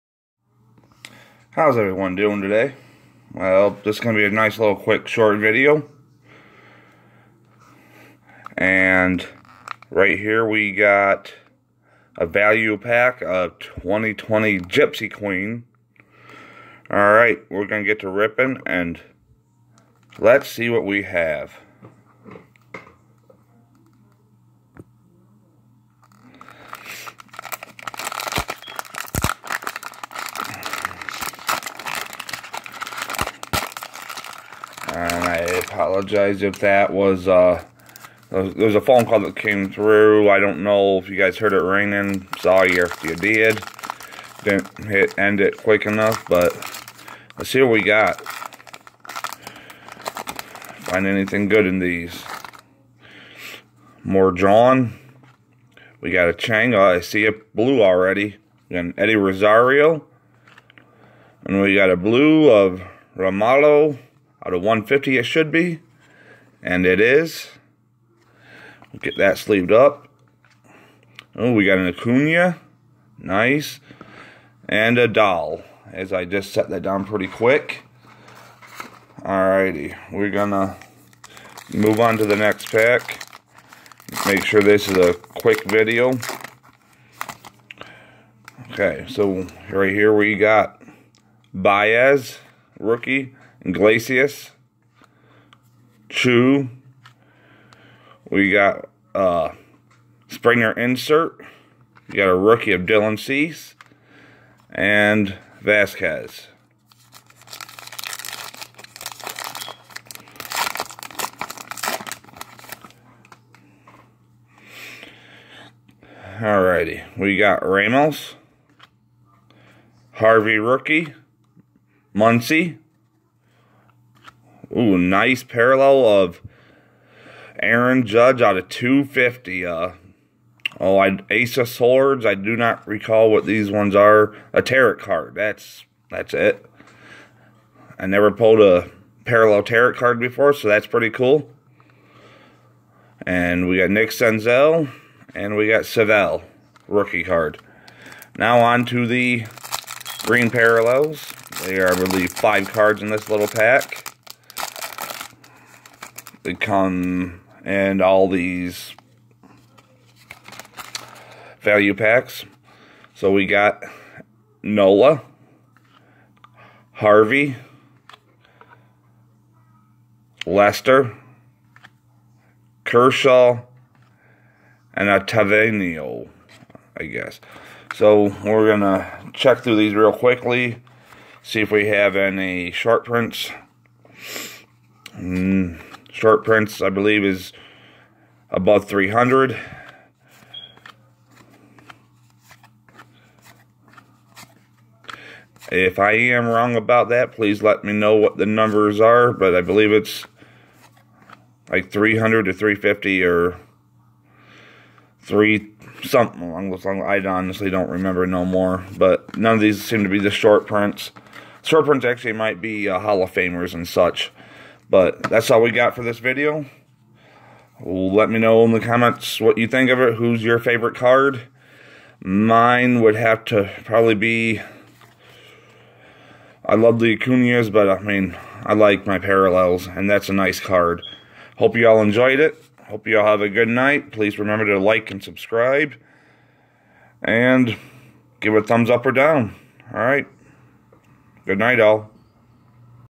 2020 Gypsy Queen
Ripping open one value pack of GQ.